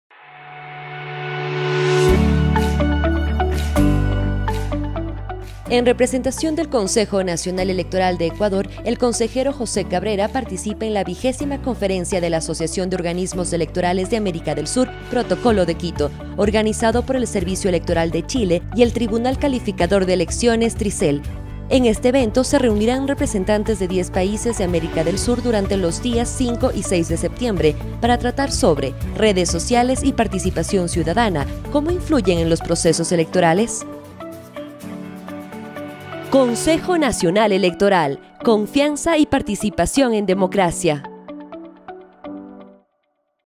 Consejero José Cabrera participa en XX Conferencia de Asociación de Organismos Electorales en Chile
En representación del Consejo Nacional Electoral de Ecuador, el consejero José Cabrera  participa en la XX Conferencia de la Asociación de Organismos Electorales de América del Sur, “Protocolo de Quito”, organizado por el Servicio Electoral de Chile y el Tribunal Calificador de Elecciones, Tricel. En este evento se reunirán representantes de 10 países de América del Sur durante los días 5 y 6 de septiembre, para tratar sobre: Redes Sociales y Participación Ciudadana ¿Cómo influyen en los procesos electorales?